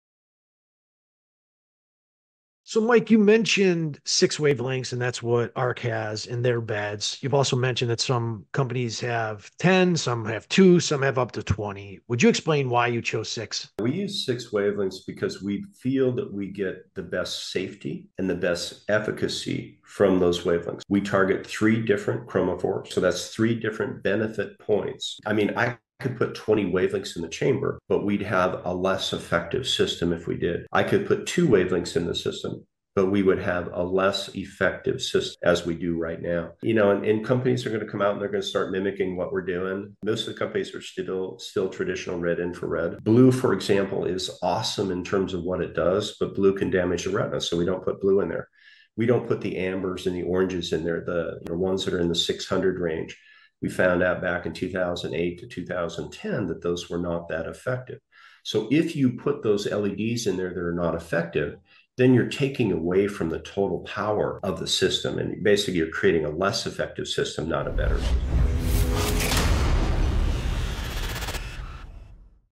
Why ARRC LED Uses 6 Wavelengths in Red Light Therapy Beds?
In this video, Mike McIntyre reveals why ARRC LED incorporates six specific wavelengths in their Red Light Therapy (RLT) beds. Discover the science and benefits behind this innovative approach and how these wavelengths work together to enhance the effectiveness of RLT. Mike explains the unique advantages of using multiple wavelengths and how ARRC LED is leading the way in advanced light therapy technology. Learn why this multi-wavelength strategy sets ARRC LED apart in the industry.